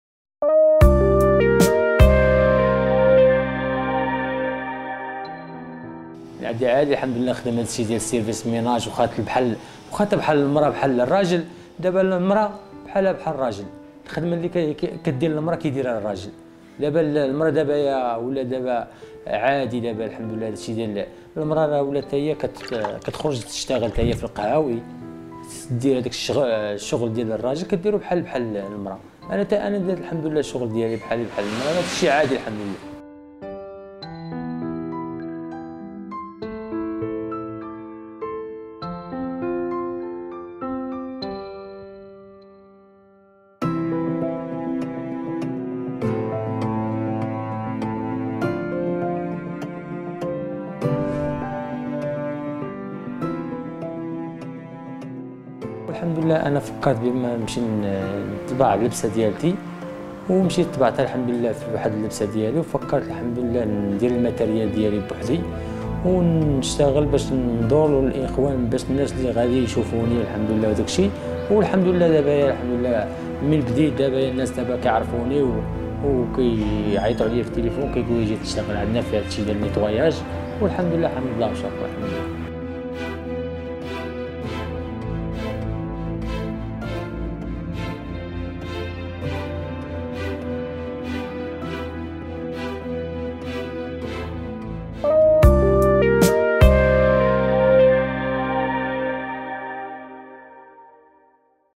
"أنا فشكل".. سفيان الريني شاب مغربي يعرض خدماته في "الميناج" بكل فخر واعتزاز
إعداد : أميمة لبيض
إخراج : مهدي فرحي